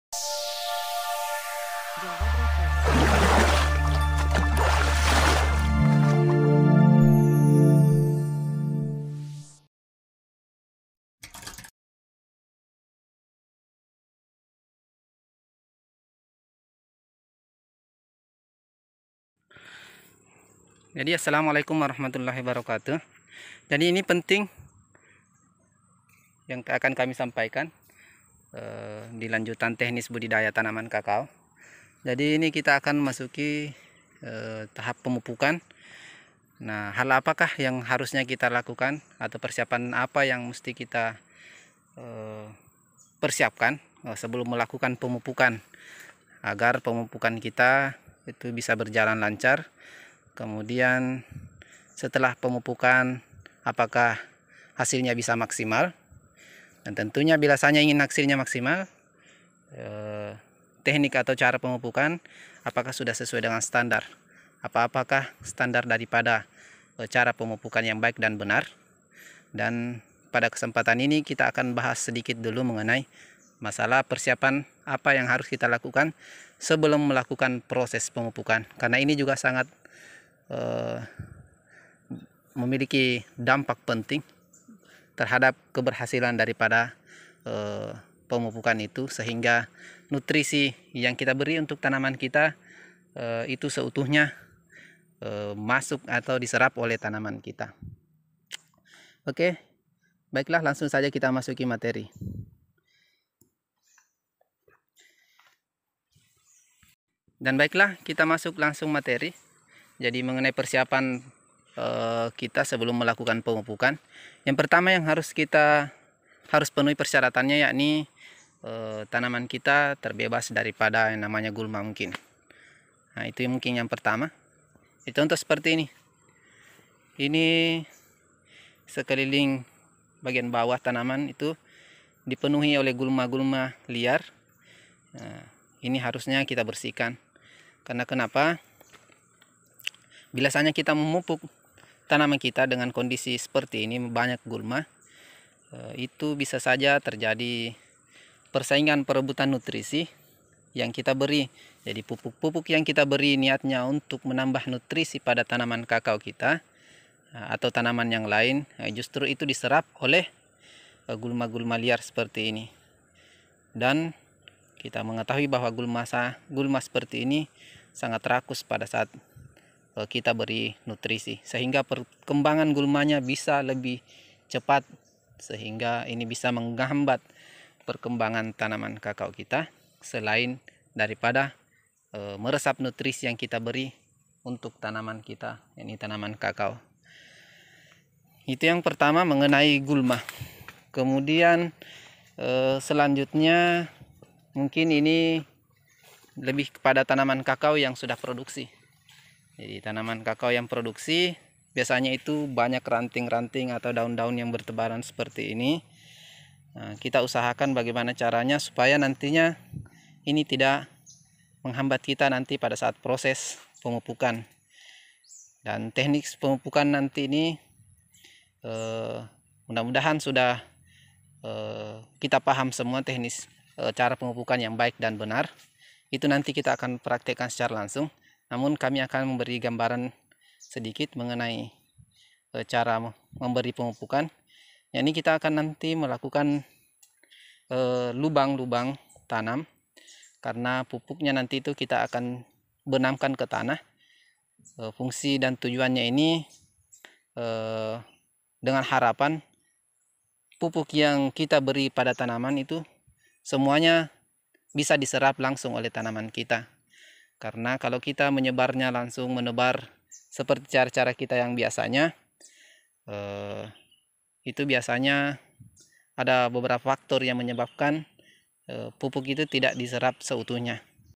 Persiapan Sebelum Pemupukan
assalamualaikum warahmatullahi wa barakatuh sobat tani semua
kini tiba masuk masa pemupukan olehnya demi materi terperinci kami akan buat beberapa part Krn materi ini merupakan salah satu yg terpenting dalam teknik budidaya tanaman
Part 1 persiapan sebelum Pemupukan
part 2 mengenal jenis pupuk
part 3 kandungan dan fungsi pupuk
part 4 cara pengaplikasian pupuk
part 5 Pupuk dulu Atau pangkas dulu